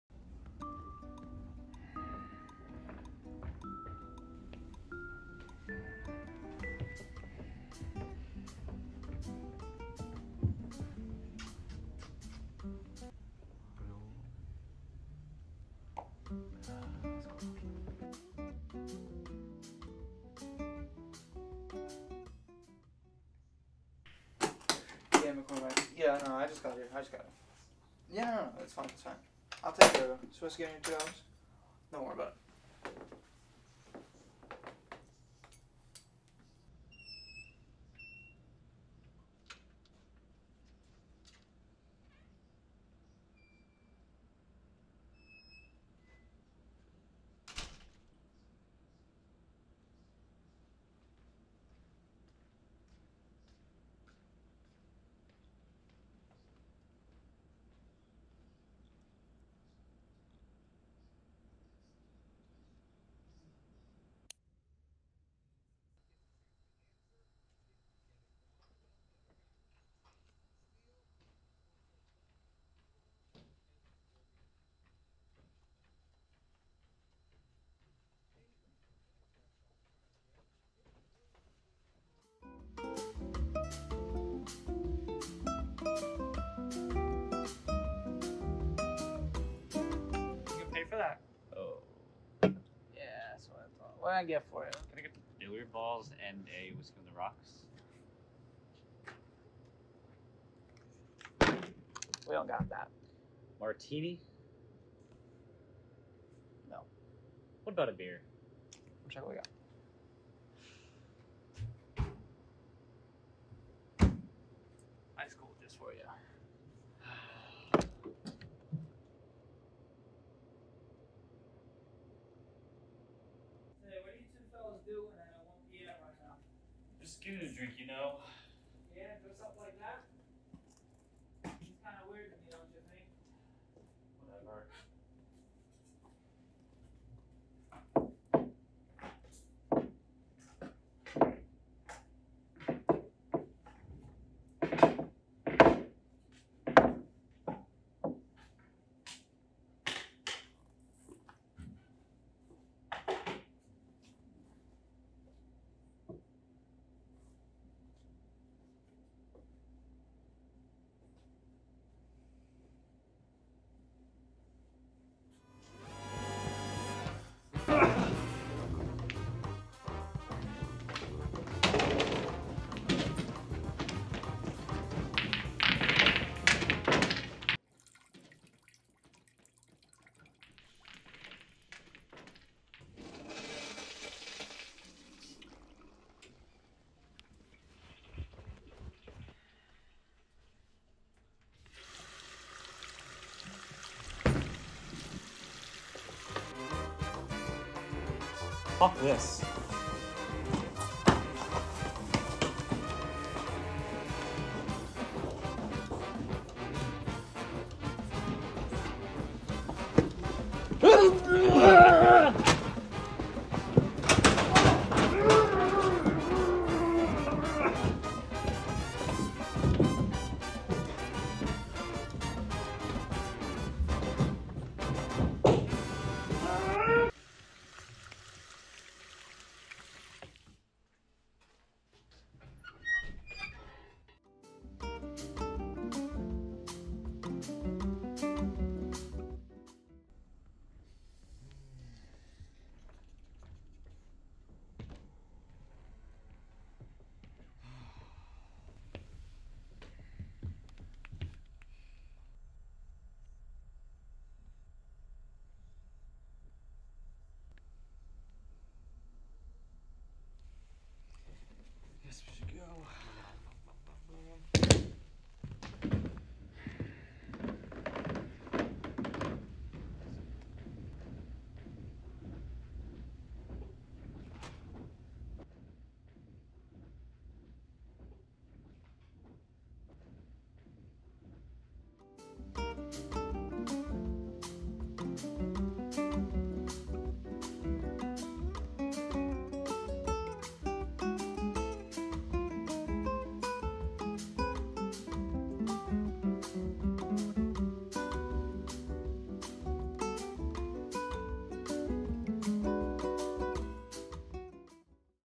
Polonia - A Short Fight Scene
Hey, guys :) I'm back to showcase a fight sequence I worked on. We will be going back in the summer to reshoot a short film.